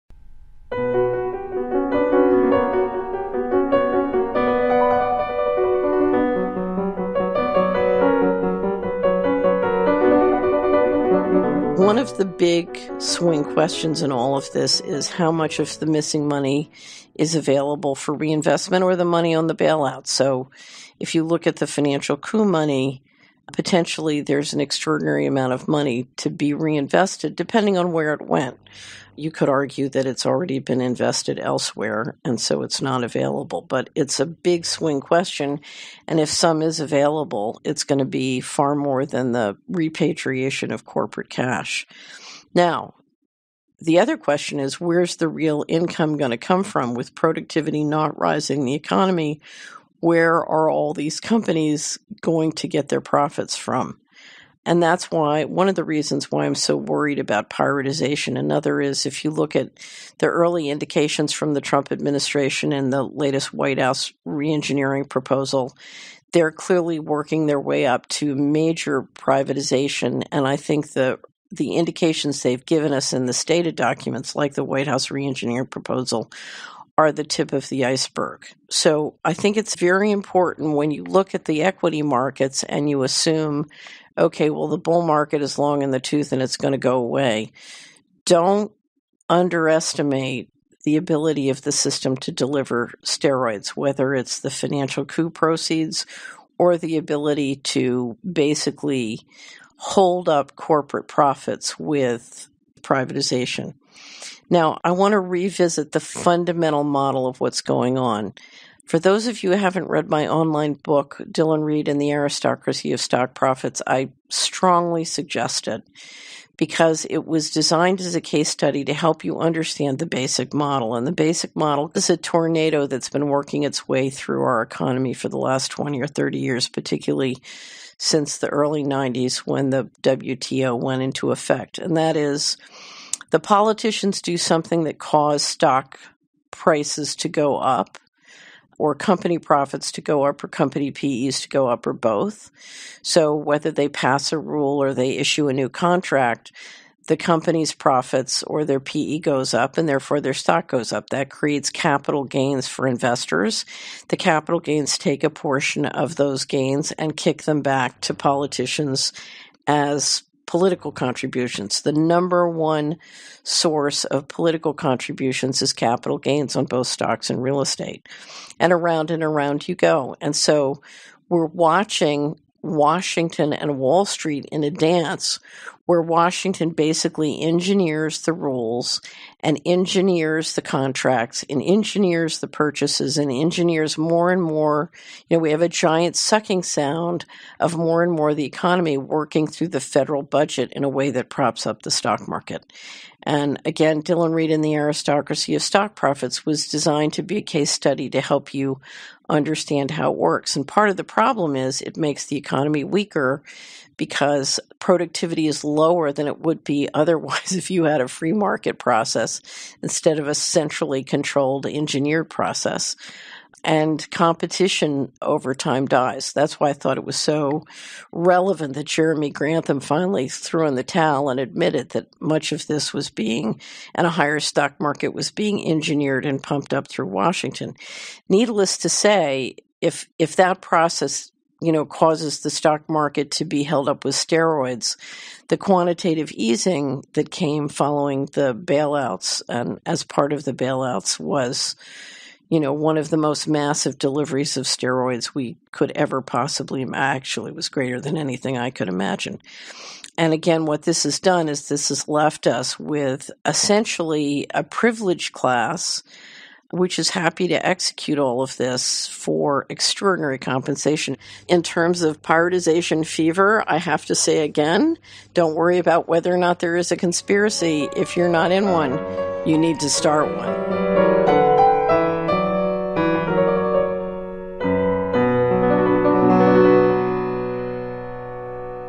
3rd Quarter 2018 Wrap Up Equity Overview
This week on The Solari Report, I will present the 3nd Quarter 2018 Wrap Up Equity Overview, including the new Rambus Blockbuster Chartology. Make sure to check out the financial charts on our 3nd Quarter 2018 Wrap Up web presentation and the Chartology when it is posted on Thursday.

I want to revisit the fundamental model operating in the global financial markets. The US has now made a significant public commitment to a financial systems based on “neverland” accounting. The federal finances are non-transparent as a matter of official policy at the same time that ever more resources are centralized and directed through the federal budget and credit. Furthermore, he federal government can waive corporate transparency in the interest of national “security” while it engineers corporate profits. This means the corporations that run — and even control – the US government operations also enjoy “neverland accounting.”

We have reached the point where stock and bond market prices are politically determined and subsidized with citizens funds – including taxes, savings and pension funds. If prices and financial disclosure are more or less meaningless, and Constitution and financial management laws are suspended, how do we navigate? How do we save and invest? How do we view the global securities markets?

I will touch on the most important events in the global equity markets since our last Equity Overview:

    Divergence between US and global markets
    Where are we -correction or bear market?
    AI Superpowers – the billion dollar IPOs
    The Global 2.0-3.0 divergence